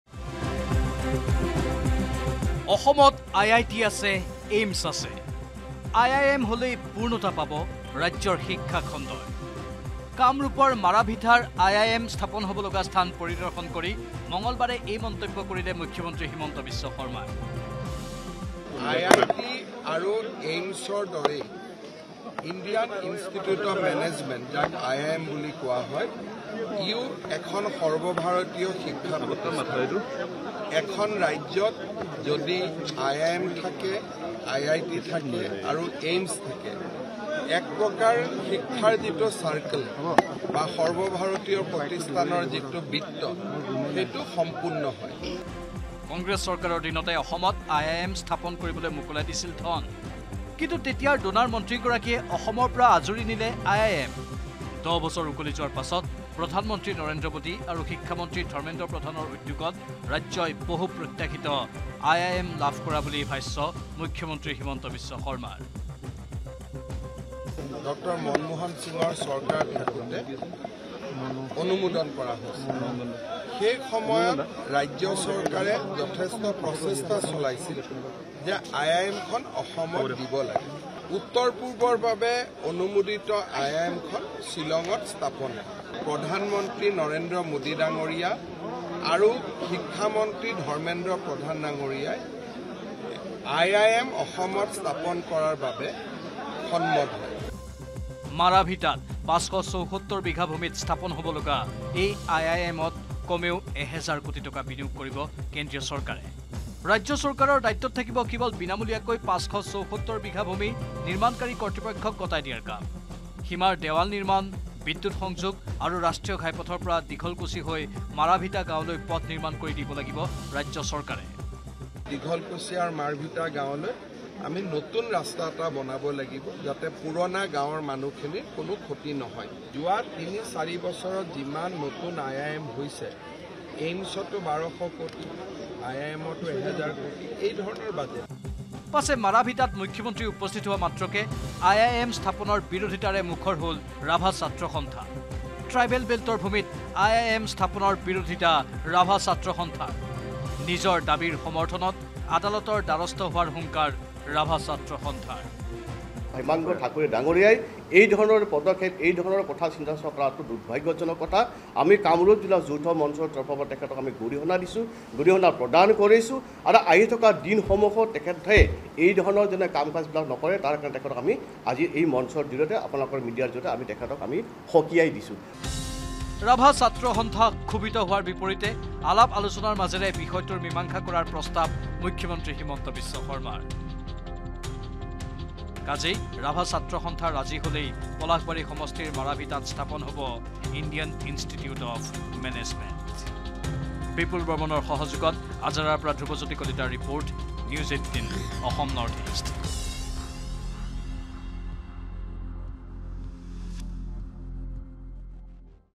Assamত নতুনকৈ প্ৰতিষ্ঠা হ’বলগীয়া IIMৰ স্থান পৰিদৰ্শন কৰে মুখ্যমন্ত্ৰী হিমন্ত বিশ্ব শৰ্মাই N18V
Assamত নতুনকৈ প্ৰতিষ্ঠা হ’বলগীয়া IIMৰ স্থান পৰিদৰ্শন কৰে মুখ্যমন্ত্ৰী হিমন্ত বিশ্ব শৰ্মাই